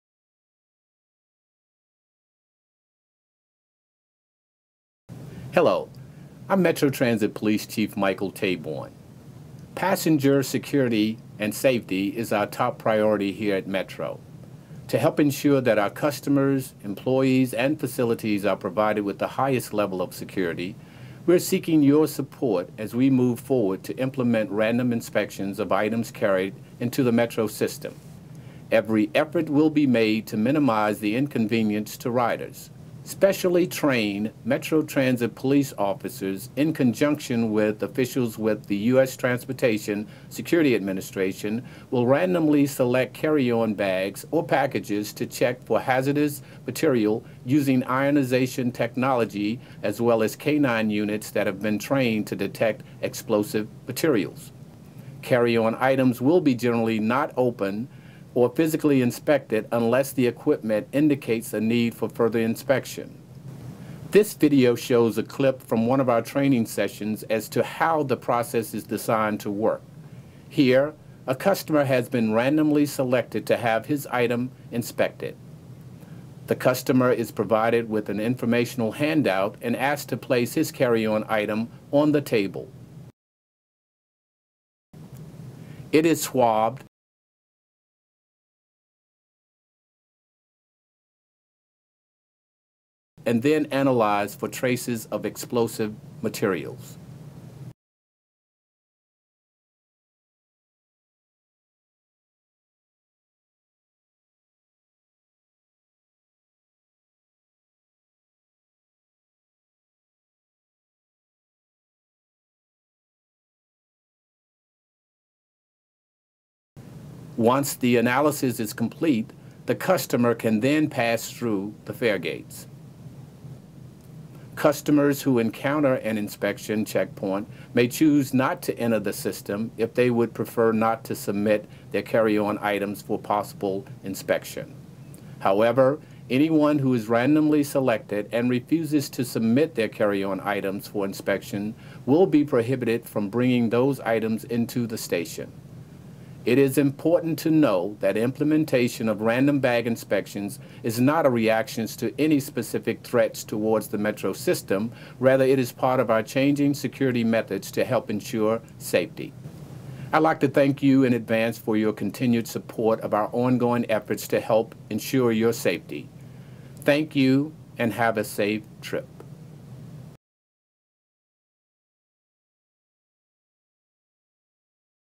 Random Bag Inspections
Metro is seeking your cooperation with our increased security efforts. Metro Transit Police will be performing inspecitons of carry-on items throughout the system.